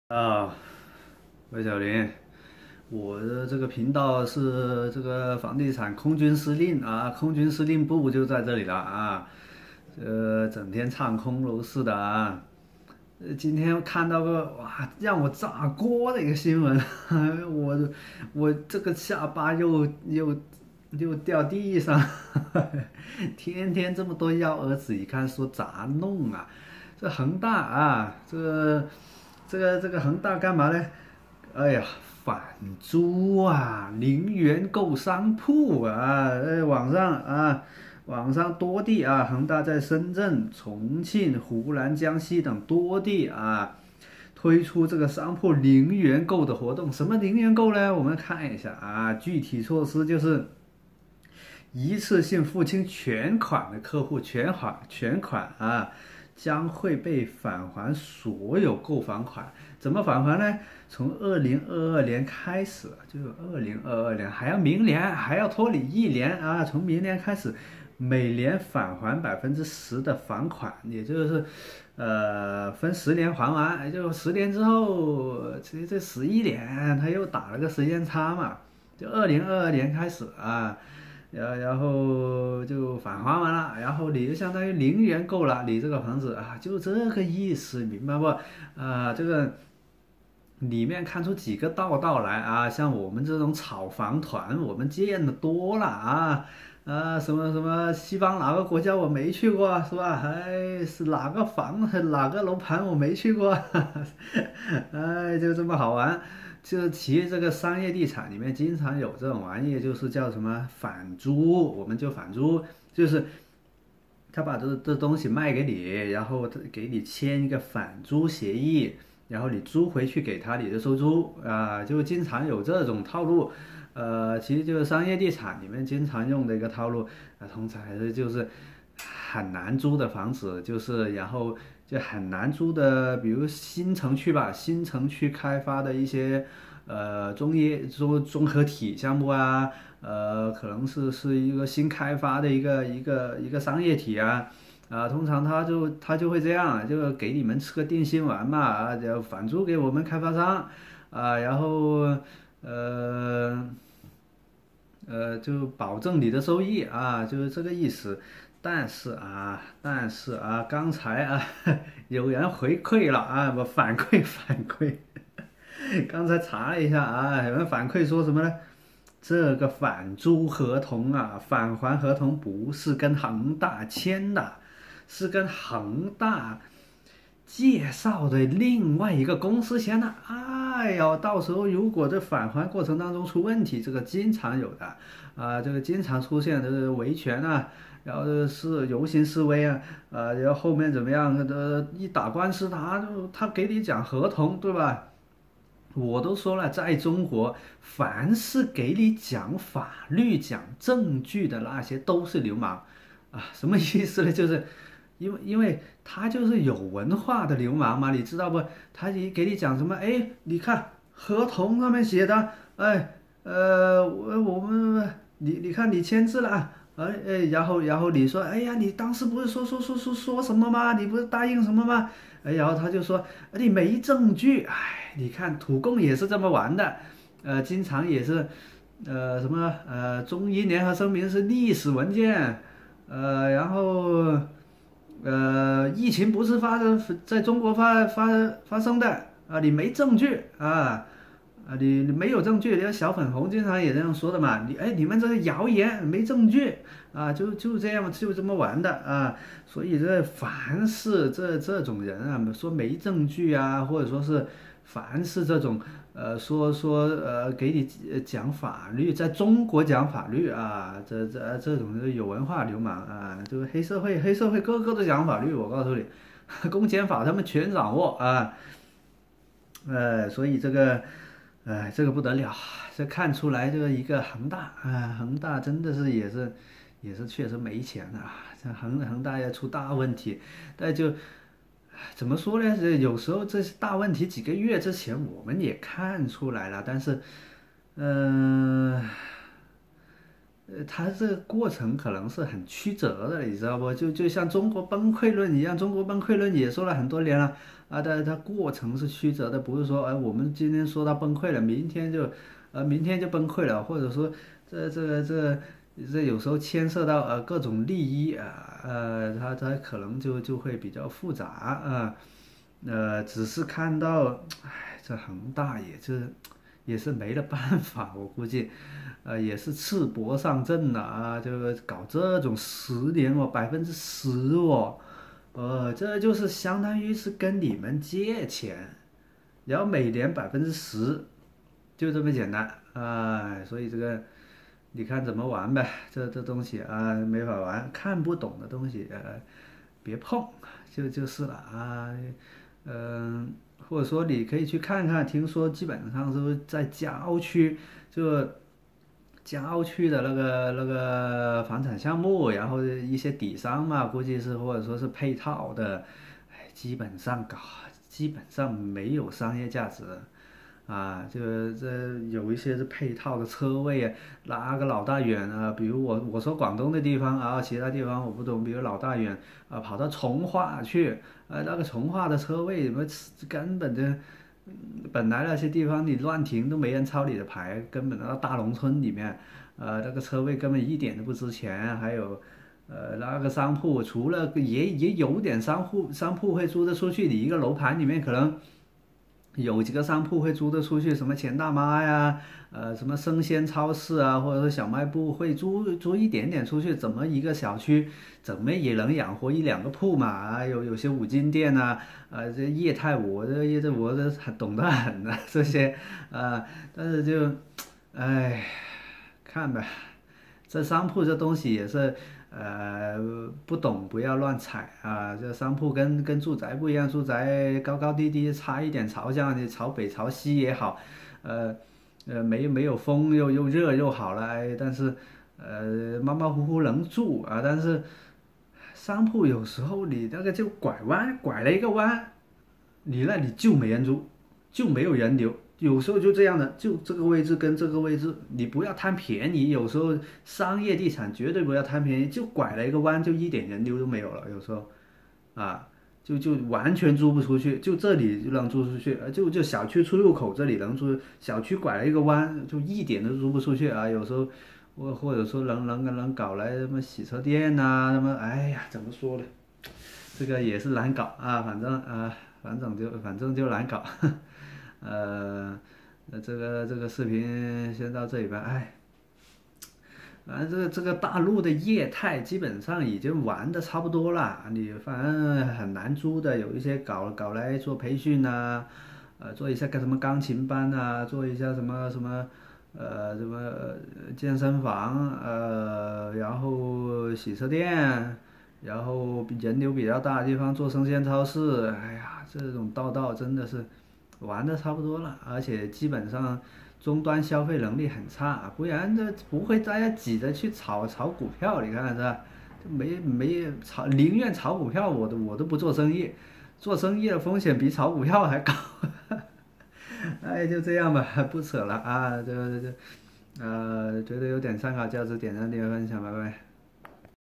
恒大赤膊上阵！0元购商铺！实为高额返租骗局，这究竟得有多缺钱啊？
恒大名声基本上臭了，许总还要搞恒驰电动车，太好玩了